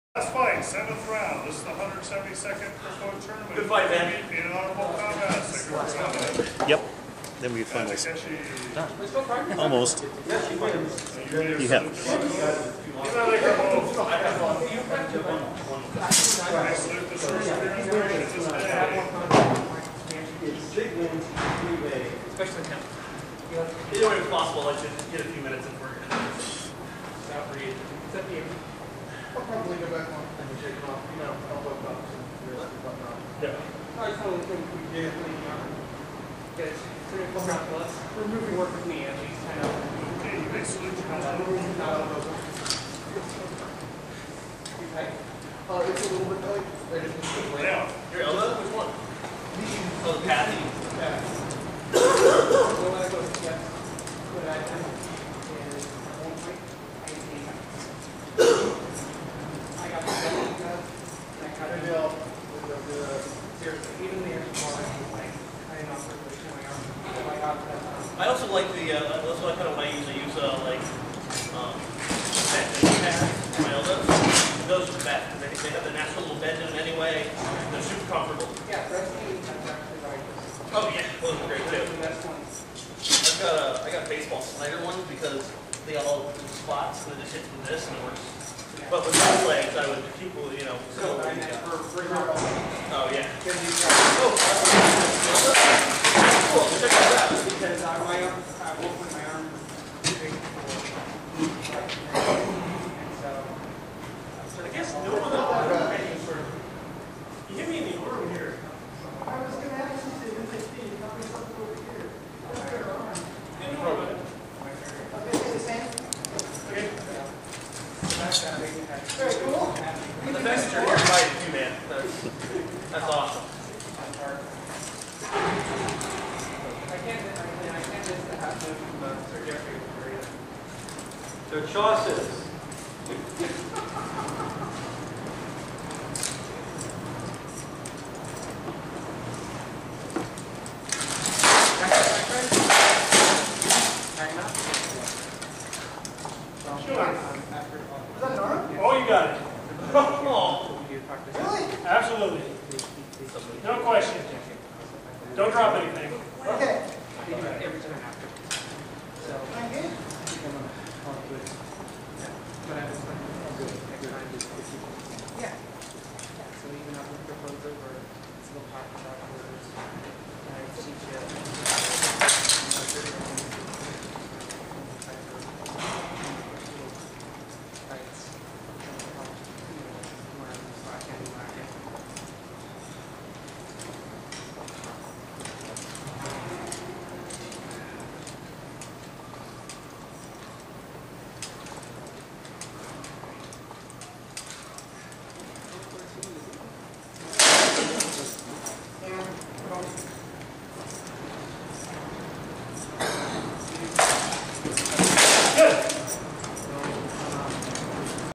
171st Crapaud Tournament, Round 7 Fight 4 Part 1.wmv
Part 1 of Fight 4 in Round 7 of the 171st Crapaud Tournament
Viscount Sigifrith Hauknefr vs Takeshi Reikoku